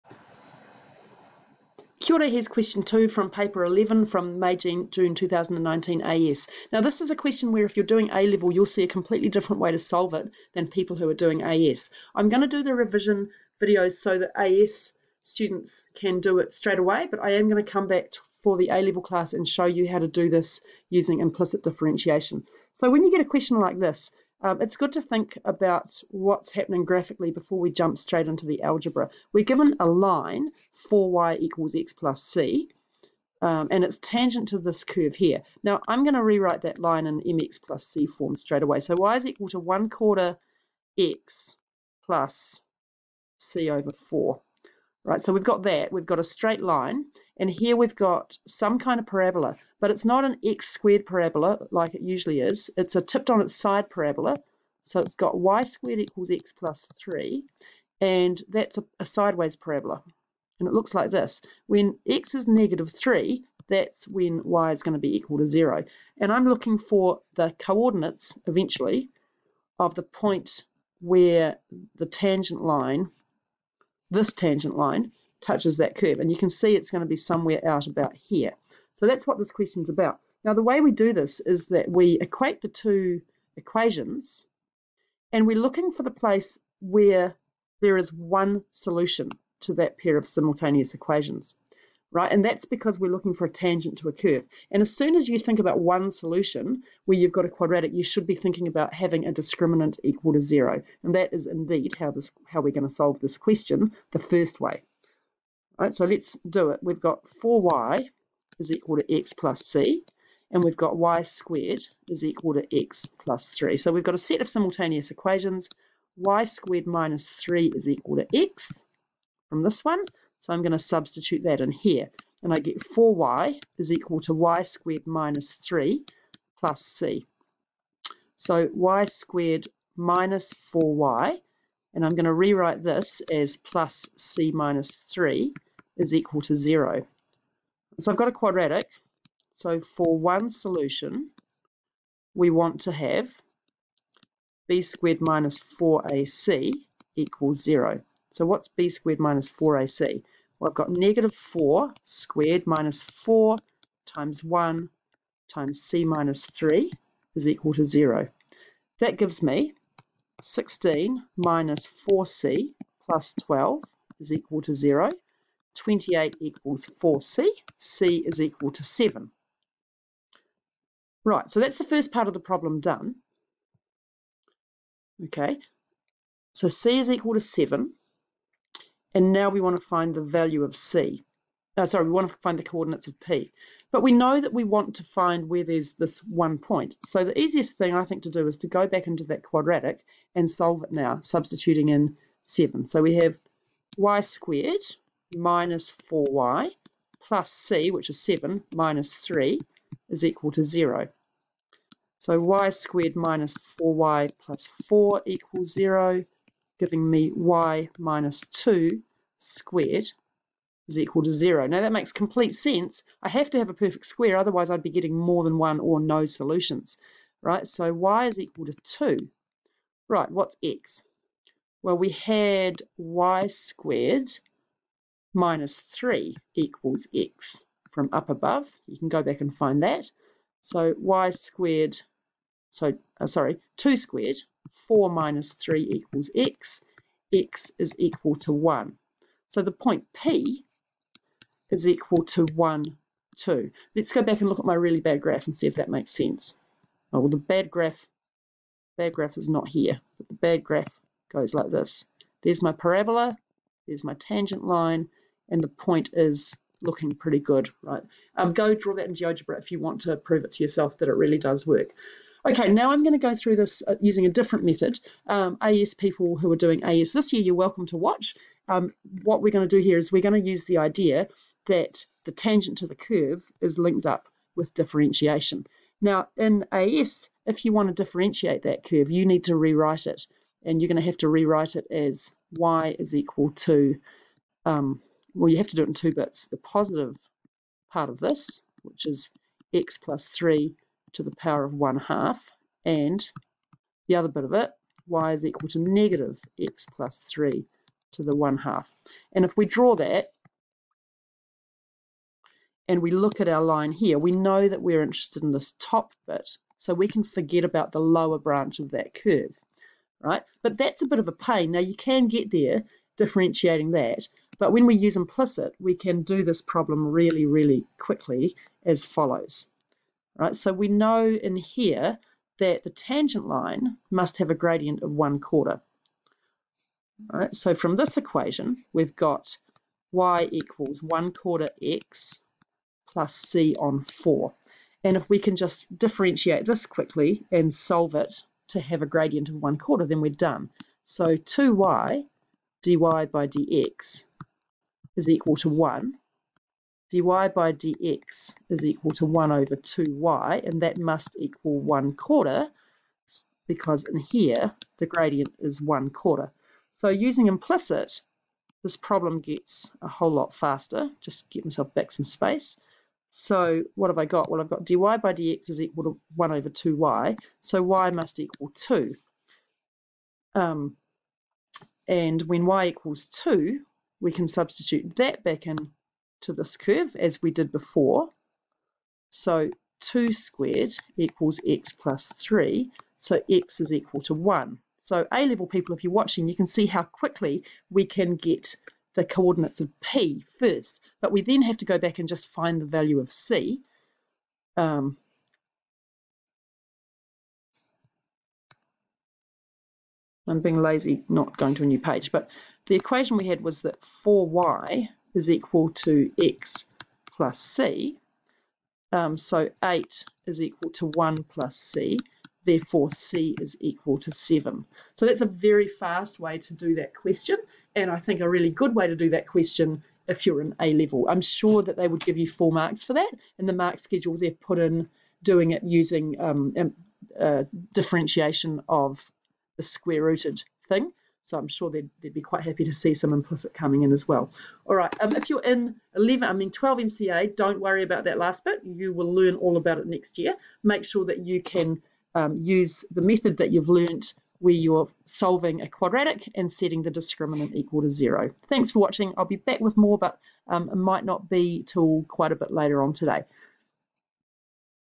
AS Revision 13MCA/12MCA May/June 2019 P11 Q2
Parabola and tangent problem. Discriminant. Also implicit if doing A level.